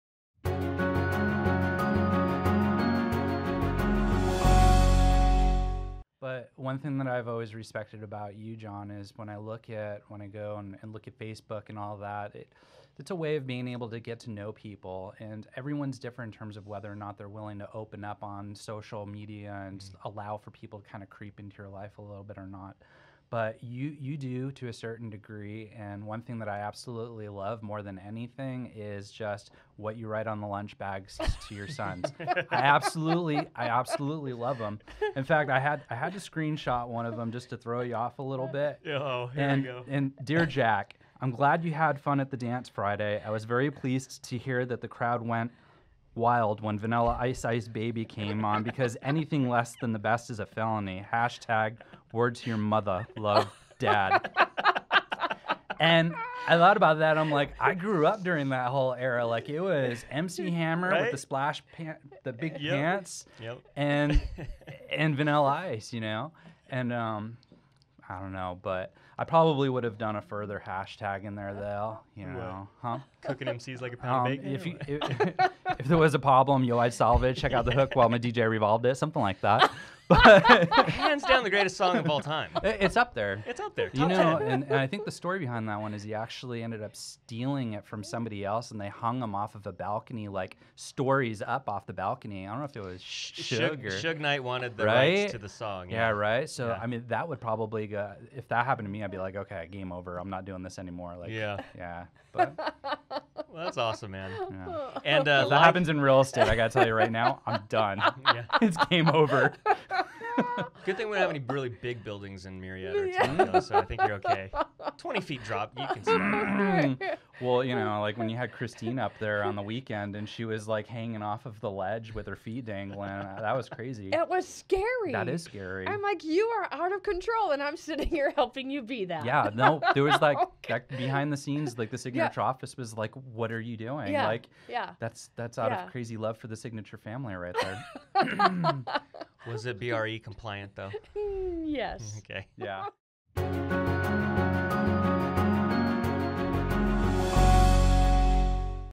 Ice Ice... Did we just become Best Friends? | Signature Spotlight Series | Red Tie Community Show
Scott and Bald Guy Red Tie share a mutual love for... Vanilla Ice?